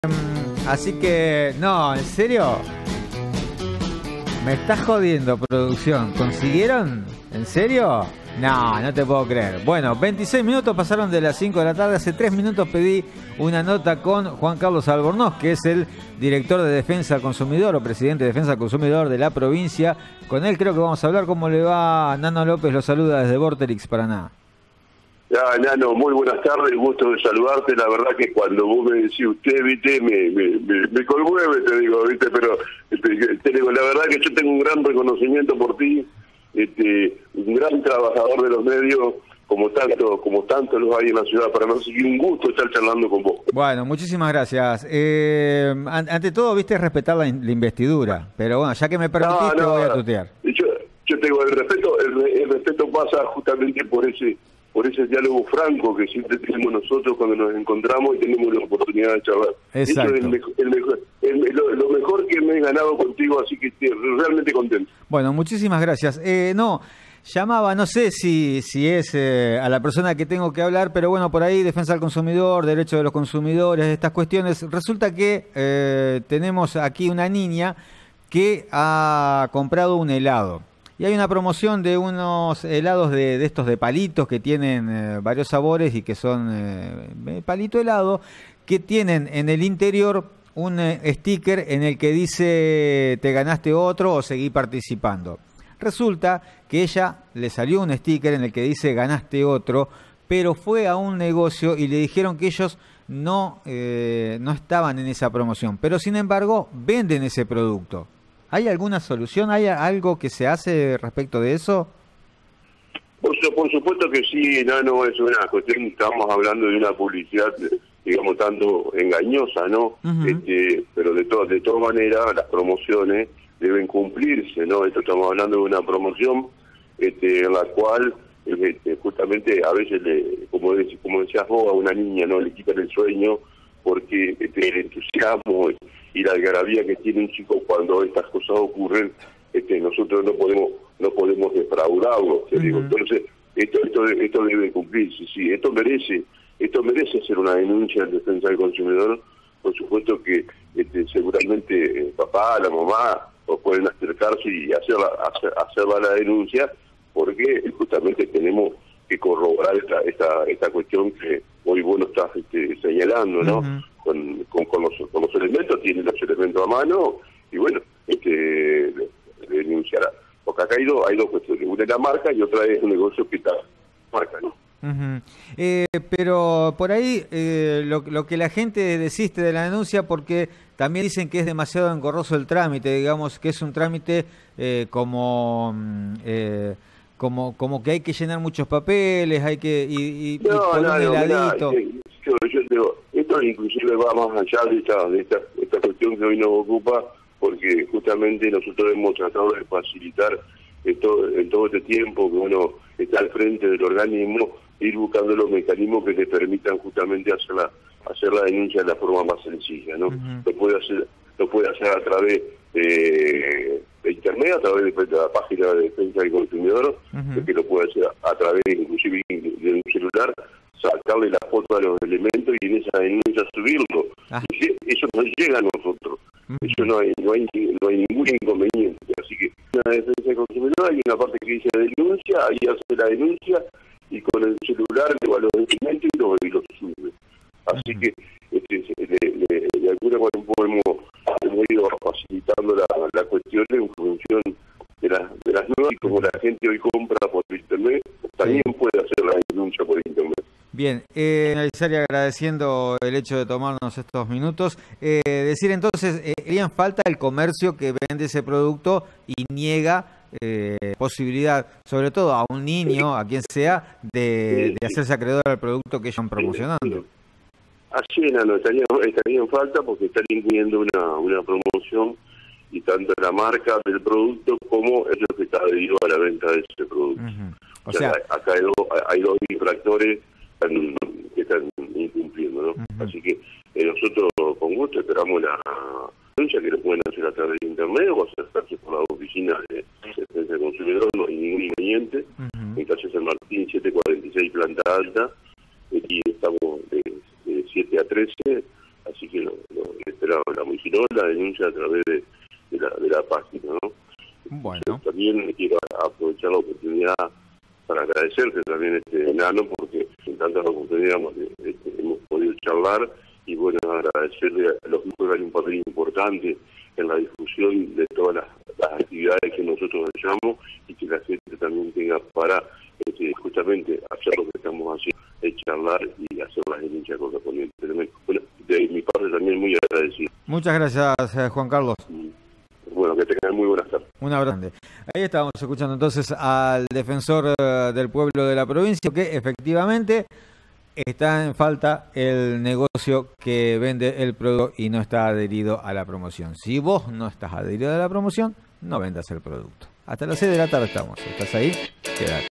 Las promos son para cumplirse
Hablamos con el defensor al cosumidor de la provincia, Juan Carlos Albornoz, ante la negativa de un negocio de reconocer a una niña el "ganaste otro" de un helado. Sostiene nuestro entrevistado que si el comercio vende el producto debe respetar la promoción del mismo.